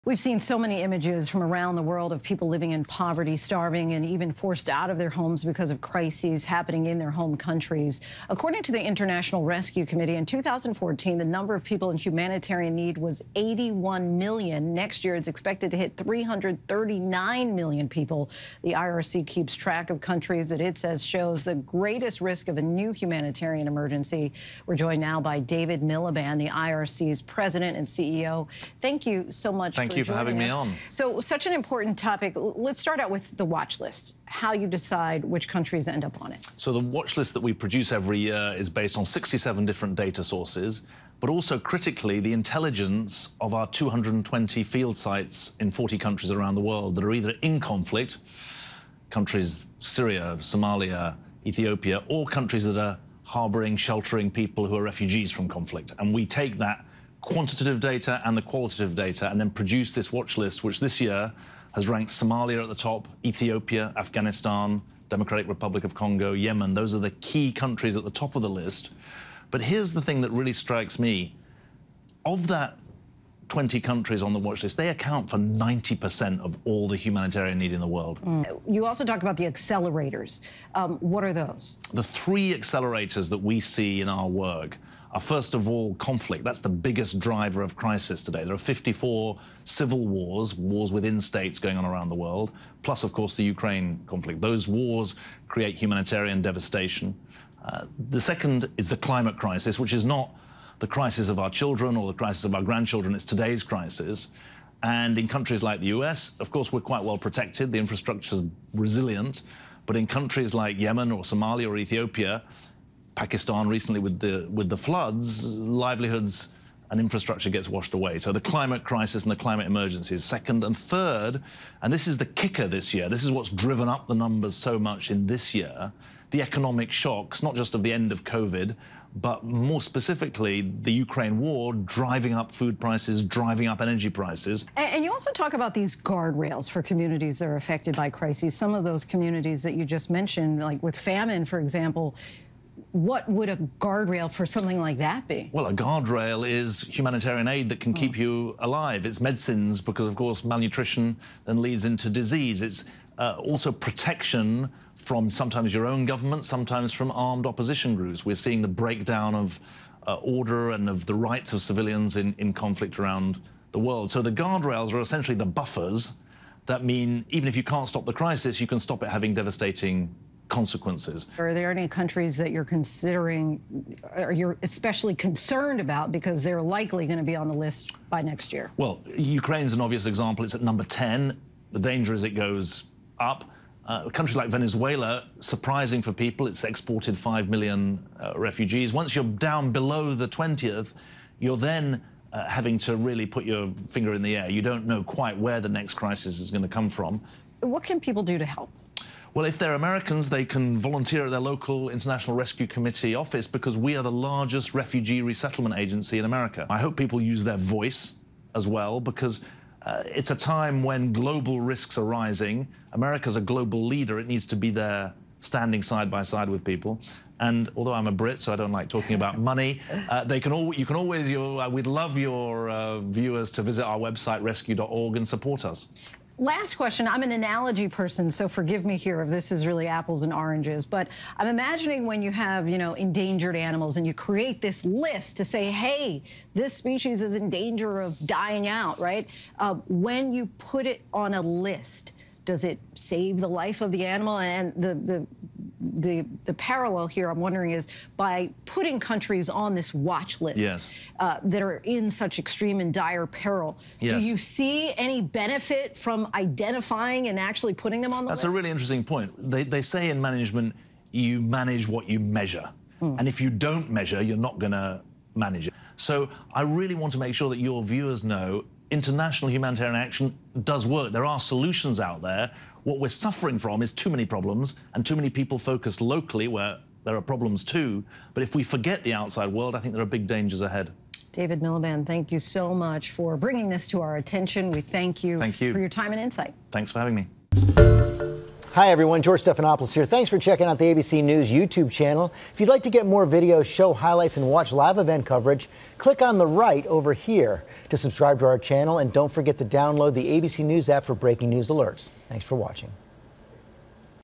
‘Humanitarian action does work’: International Rescue Committee president | ABCNL
ABC News’ Linsey Davis spoke with David Miliband, head of the International Rescue Committee, as they track the 2023 watchlist of humanitarian crises around the world.